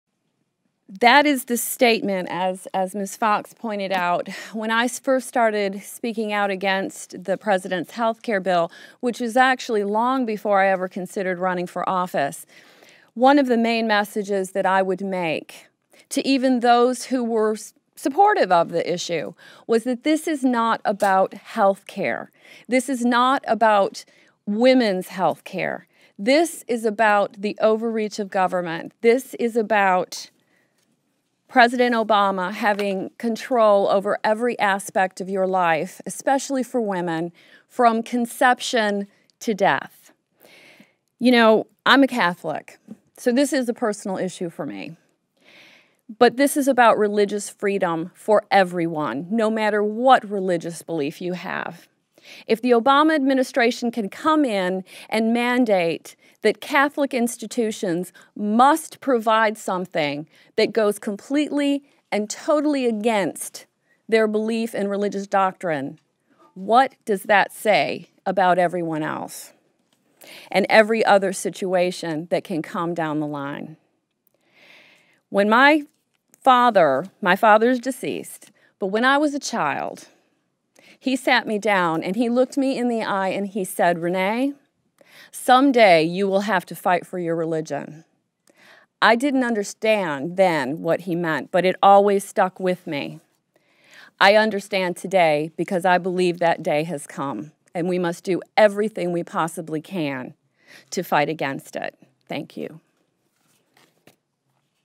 Rep. Ellmers Hosts Press Conference on HHS Mandate
Representatives Renee Ellmers (R-NC-2) and Marsha Blackburn (R-TN-7) hosted a press conference this morning on the Obama Administration's HHS mandate and its threats to religious liberty.